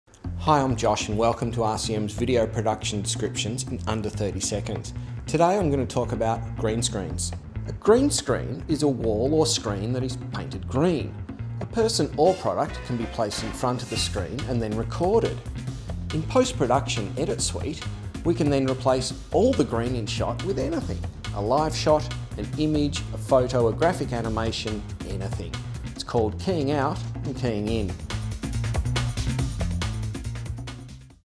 What are Green Screens? Video Production Descriptions in under 30 Seconds
A production Green Screen is a wall or screen that is painted green, a person or product can be placed in front of the screen and then recorded on video. In a post-production edit suite, we can then replace all the green in shot with anything...a live shot, an image, photo, graphic animation...anything! It's called Keying Out and Keying In